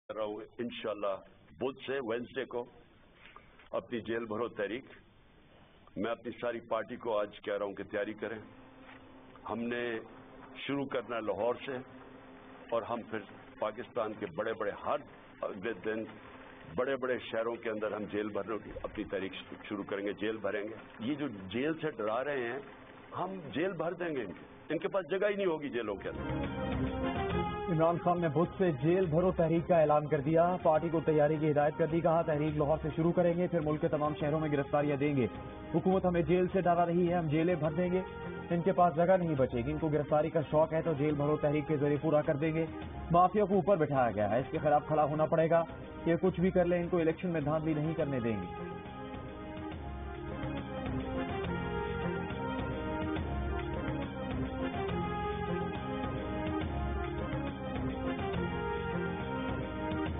PTI Chairman Imran Khan announces ‘Jail Bharo’ movement starting from Lahore.
PTI chief says it's dangerous CEC is showing inability on conducting polls.
"Caretaker chief ministers would be illegal on 91st day," claims Khan.
Blaming the incumbent coalition government for its "political victimisation" of his party leaders and allies, Pakistan Tehreek-e-Insaf (PTI) Chairman Imran Khan has announced kickstarting the ‘Jail Bharo’ (court arrest) movement from Wednesday, starting from Lahore.

"We will fill jails, they [authorities] will have no space left to hide," he said while warning the Pakistan Democratic Movement-led (PDM) government during his address to the nation via video link from Lahore's Zaman Park residence on Friday, where he has been residing since being shot in the legs on November 3 last year.

In the wake of sedition cases filed against his party leaders including Fawad Chaudhry, Azam Swati and Shahbaz Gill, Imran Khan on February 4 announced "Jail Bharo Tehreek" and asked PTI workers and supporters to prepare for movement across the country.

“They want to enslave us by threatening to put us in jails,” the former prime minister, who was ousted following a vote of no-confidence in April last year, said vowing to fulfil the wish of authorities.

Khan claimed that the police entered the homes of PTI workers in Multan and were tasked with such threatening tactics. “If this work is being done by the police, then won’t the people hate them?”